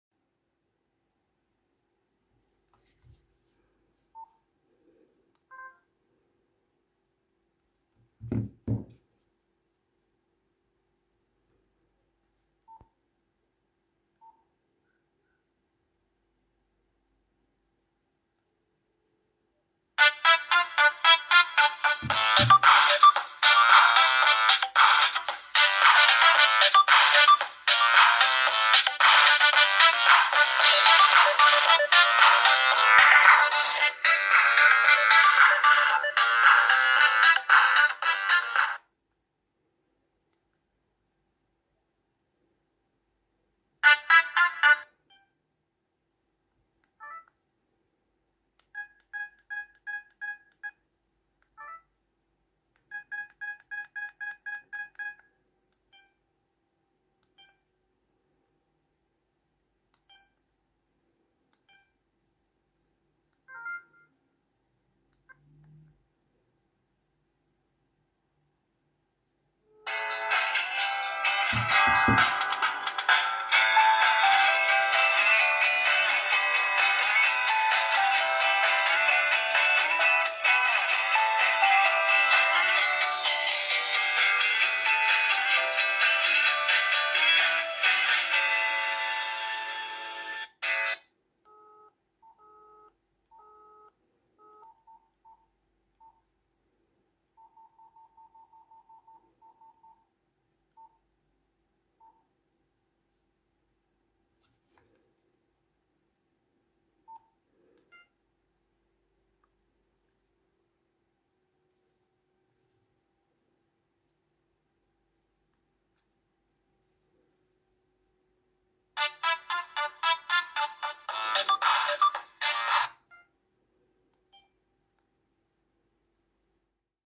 Incoming call & Outgoing call at the Same Time Nokia C3-00 + LG GW300
Incoming call Nokia C3-00
Incoming call LG GW300